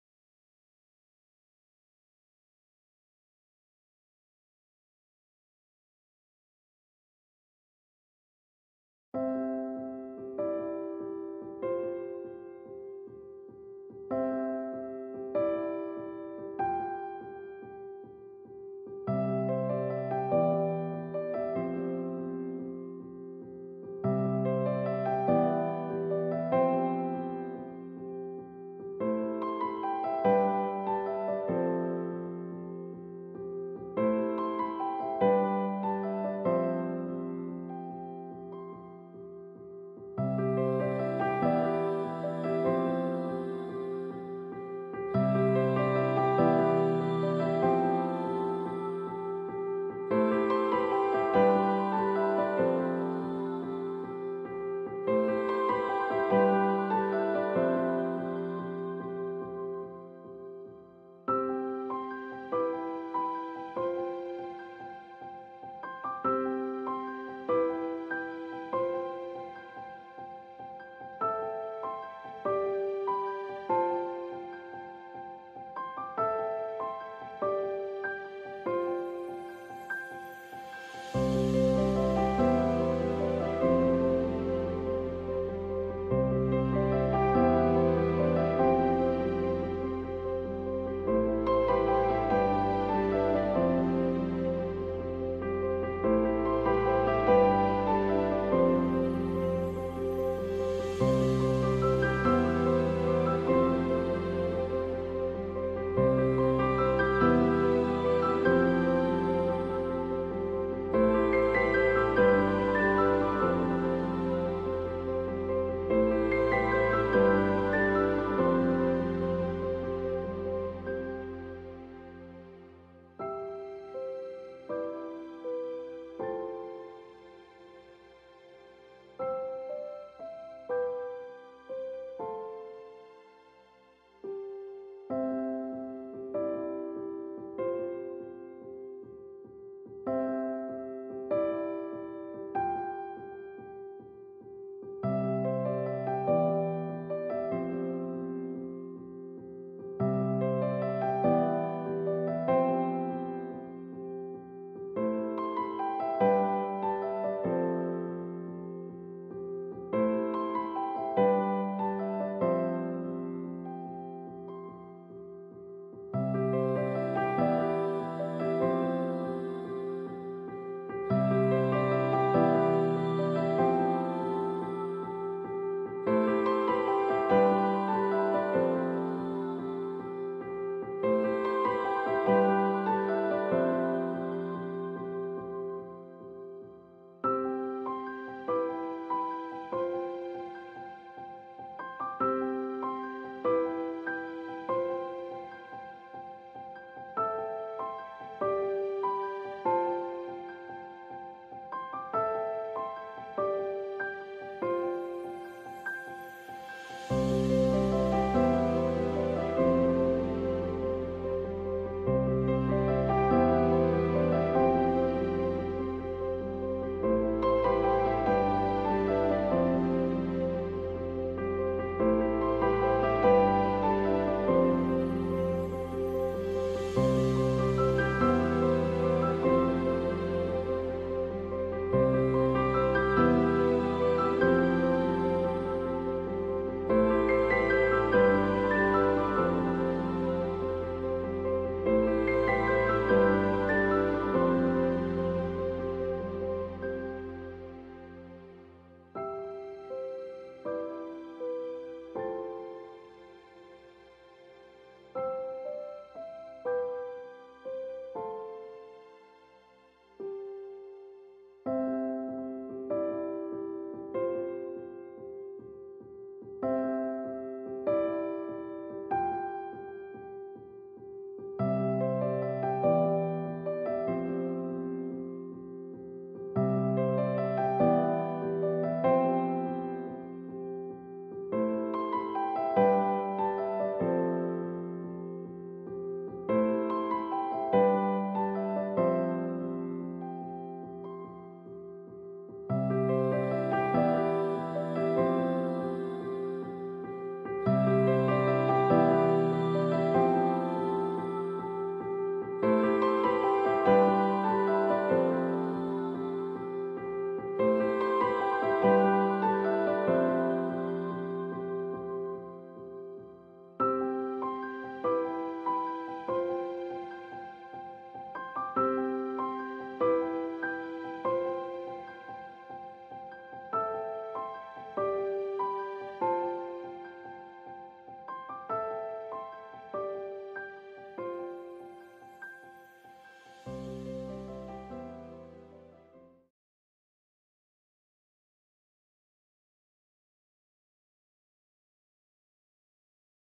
I Painted a House Sparrow: Watercolor Demo
A watercolor demo painting of a house sparrow.